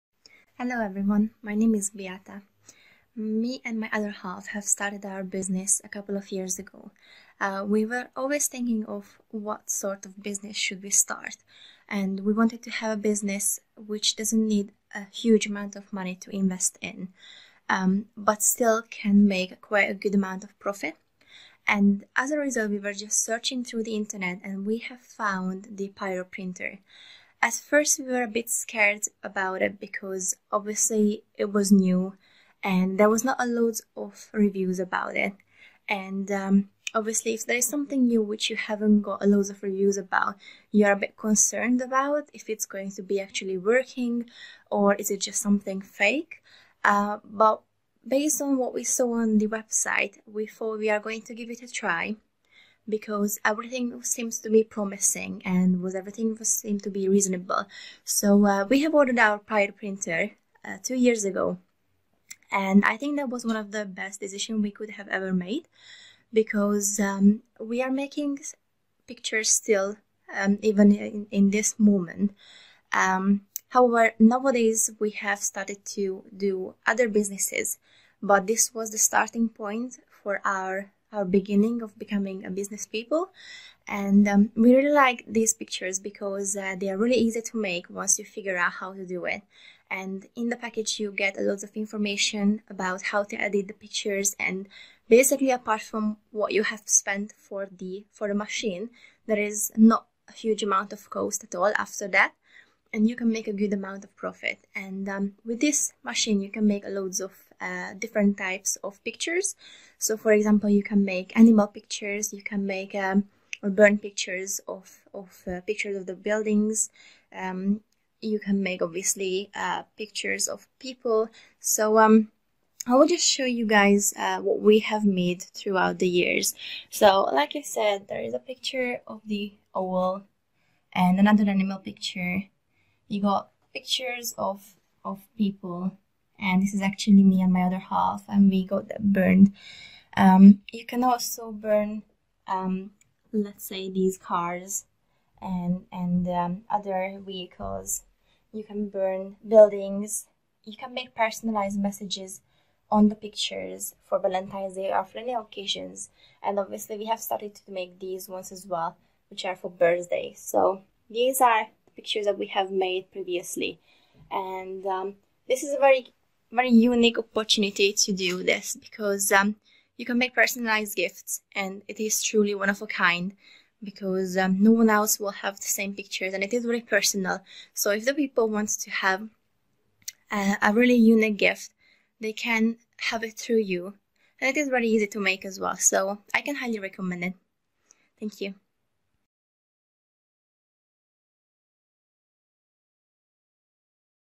Video review of Gabor Vida (UK) about Pyroprinter
Vida lives in United Kingdom. She has been using a Pyroprinter since August 2017. The CNC wood burner machine became an additional source of income for her and her husband.

This was her first experience in business, and she and her husband are still doing it as well. Now it has already opened other business areas.

In the video it shows what a stunningly beautiful picture she made with the help of this machine.